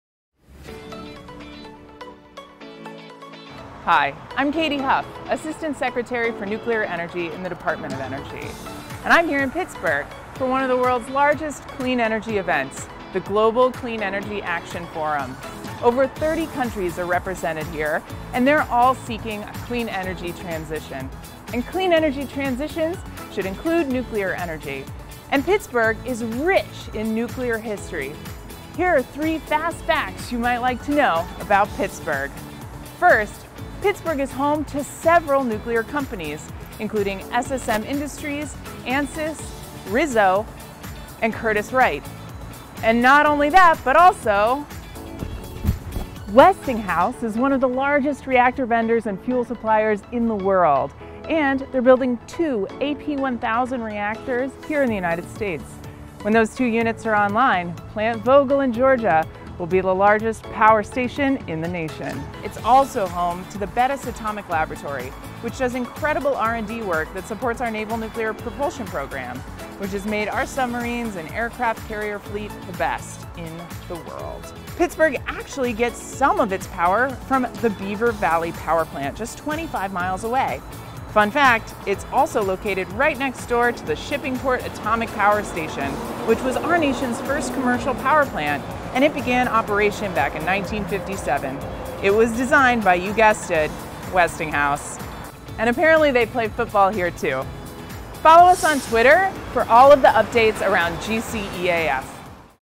3 Things Nuclear Energy and Pittsburgh Have in Common
Assistant Secretary for Nuclear Energy Dr. Katy Huff breaks down three things nuclear energy and Pittsburgh have in common at the Global Clean Energy Action Forum.